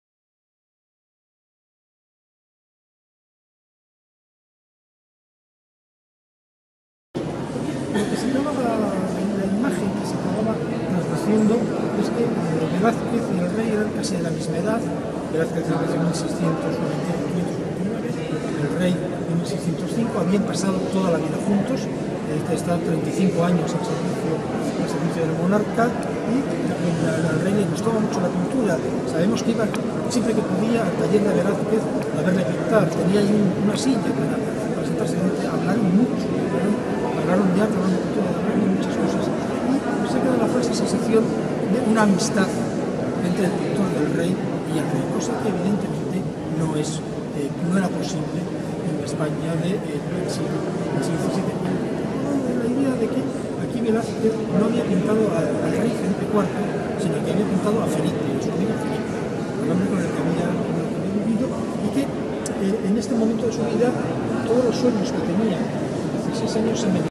Miguel Morán
Miguel Morán en la presentación de "Felipe IV. El Grande" en el Museo del Prado